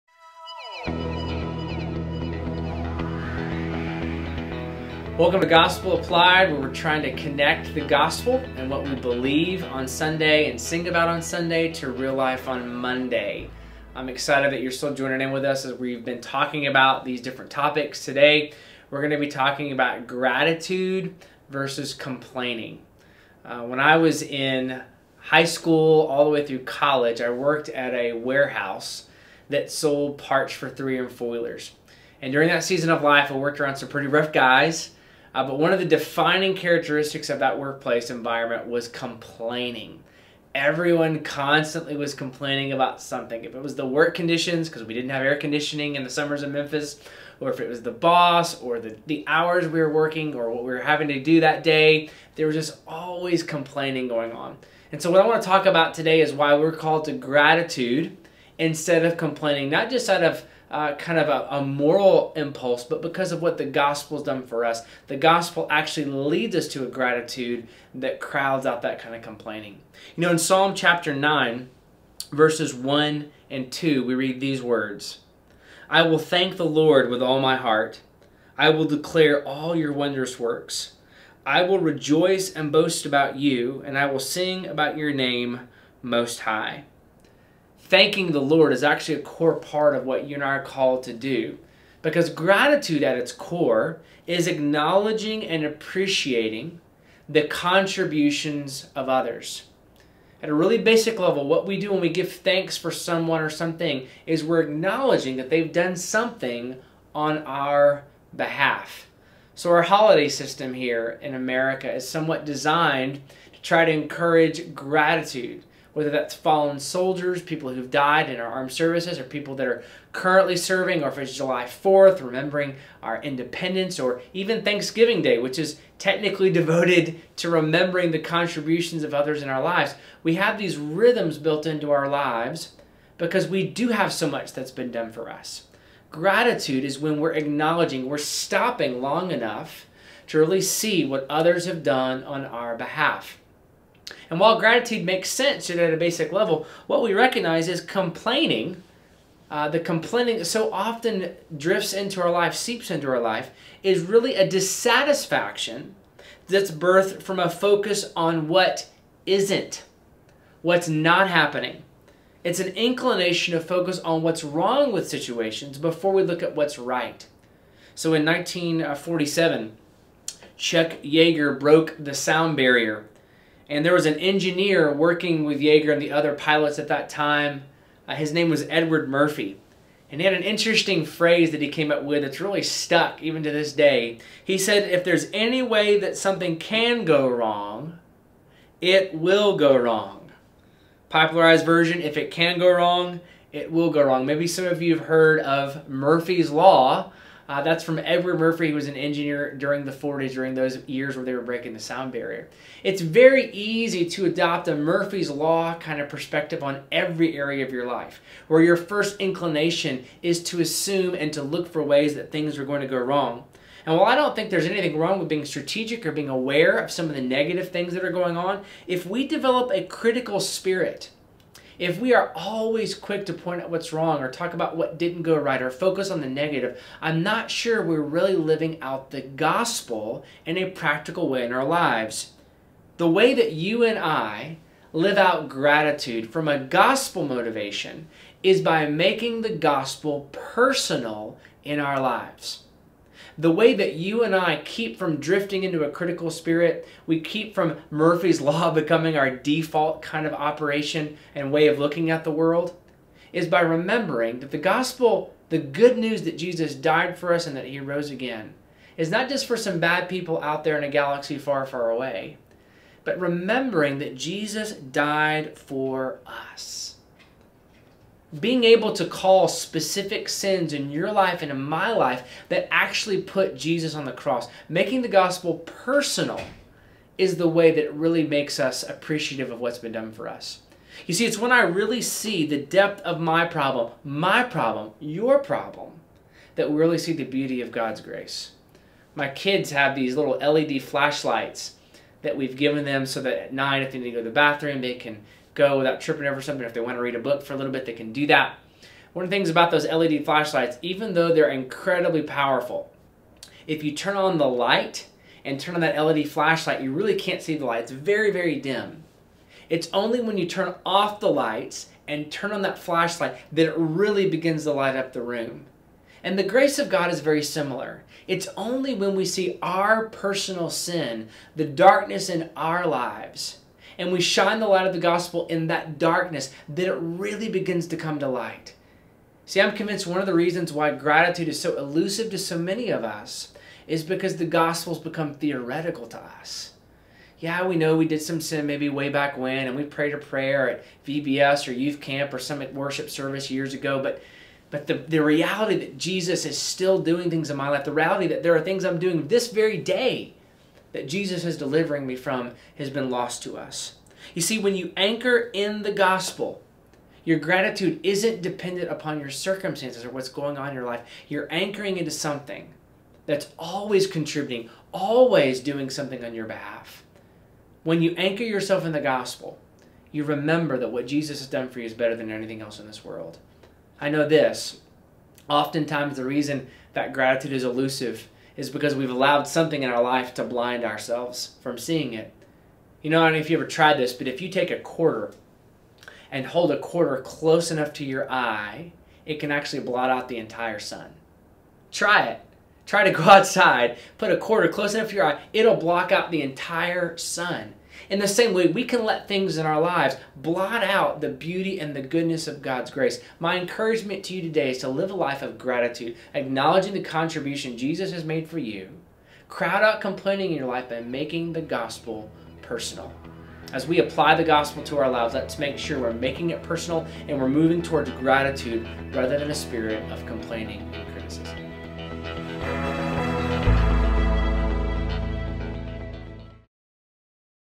Gratitude vs. Complaining
Watch as Dr. Spencer Plumlee applies what it means to live out the gospel in the area of gratitude from Psalm 9:1-2.